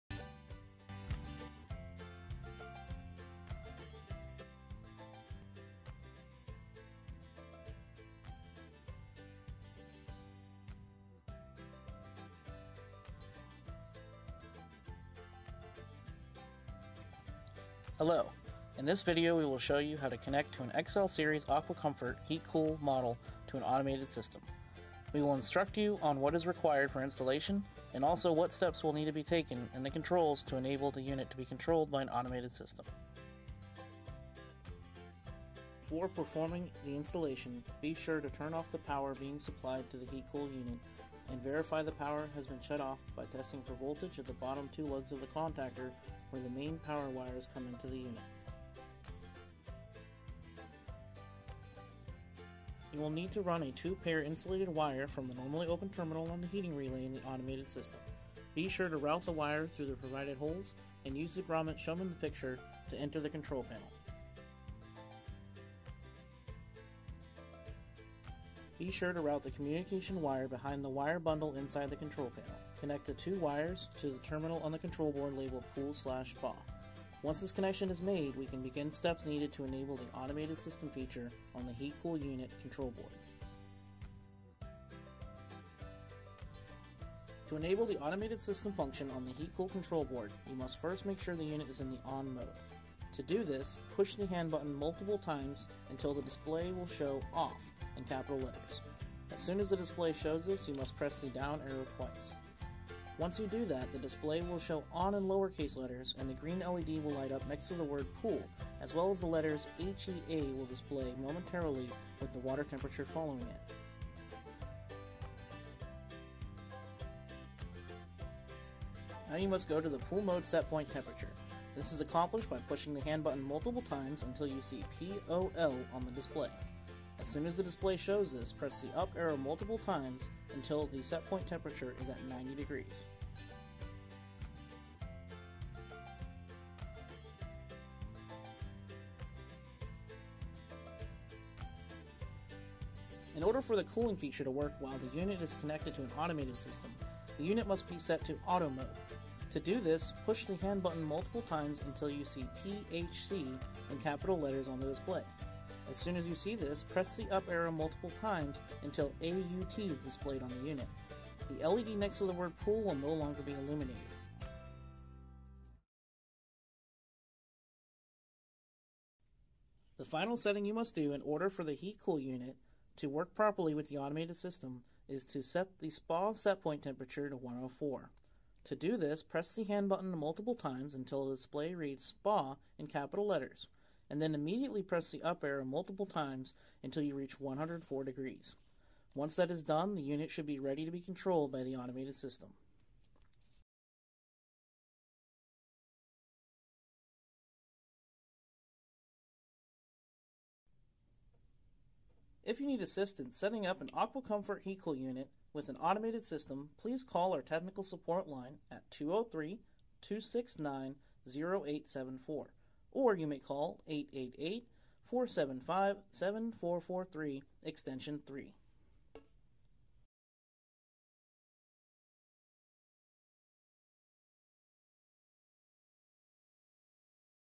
heat cool automated system instructions
In this video we instruct you how to connect and program an AquaComfort XL series Heat Cool model unit to an automated system.